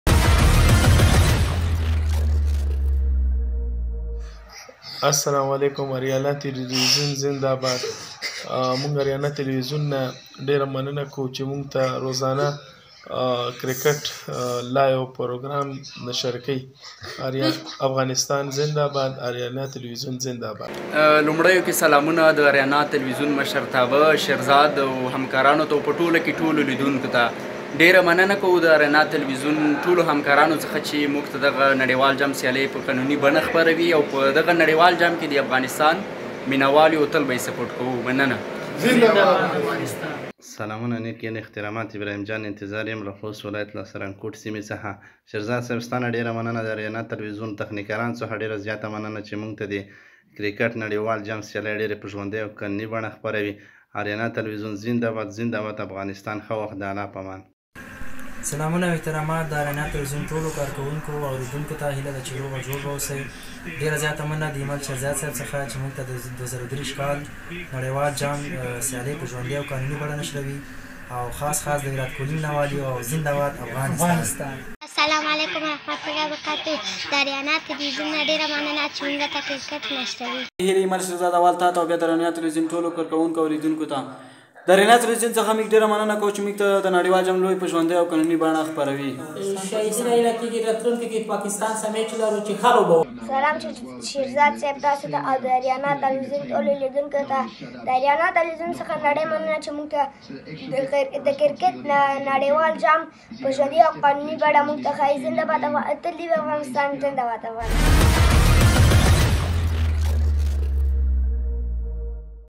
ICC Men's Cricket World Cup 2023 Fans' Message - P16/ پیام های هواداران مسابقات جام جهانی کریکت ۲۰۲۳
د ۲۰۲۳ کرکت نړیوال جام سیالۍ د مینه والو پیغامونه - ۱۶ برخه 🏏☺️🥰
پیام های هواداران مسابقات جام جهانی کریکت ۲۰۲۳ - بخش ۱۶ 🏏☺️🥰
ICC Men's Cricket World Cup 2023 Fans' Message - Part 16🏏☺️🥰